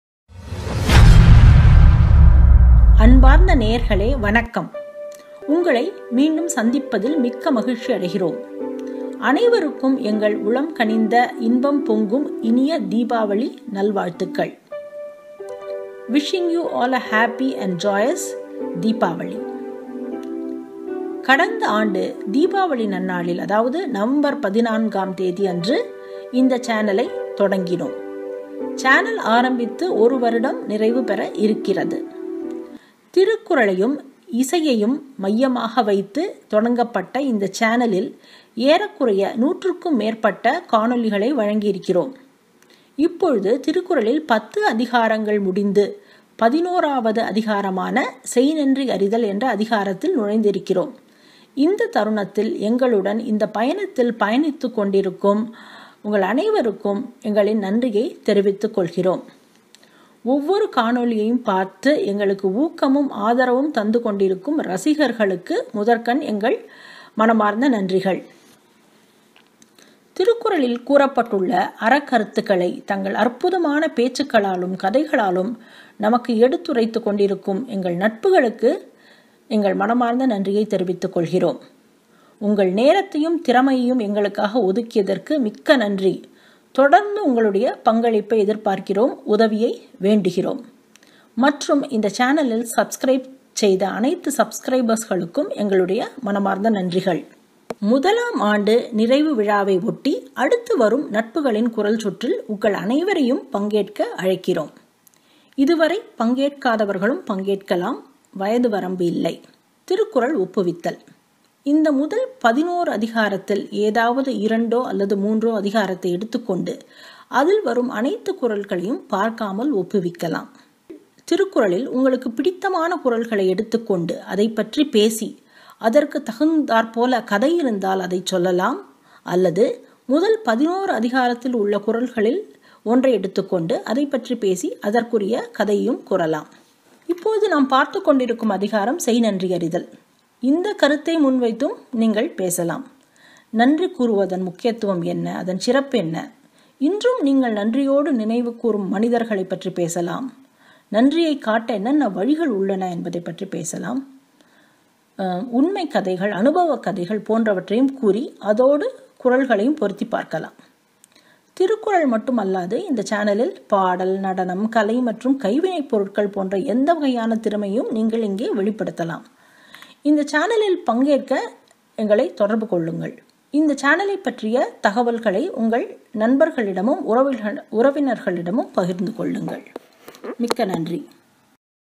Happy Deepavali 1st year anniversary A big thanks to all the viewers, subscribers, and contributors.
Last year on this auspicious day of Deepavali, we started our channel Thirukkuralum kuralum. We are almost completing one year with this channel.  With all your support and encouragement, the journey has been amazing so far.  We want to use this opportunity to express our heartful gratitude to all the viewers, subscribers, well-wishers, contributors from the bottom of our hearts.  Without you, the journey would not have been possible.  This channel is entirely run by volunteers.  Many had contributed their time and talent towards this mission without any expectations. We are aptly in chapter 11 (which is  Gratitude). There can never be a better time to say thank you than this.
Natpugalin Kural segment is open for everyone. We are calling for entries for the next segment.  Anyone having a flair for Thirukkural can participate.  Entries are accepted both in English and Tamil.  There is no age limit to participate.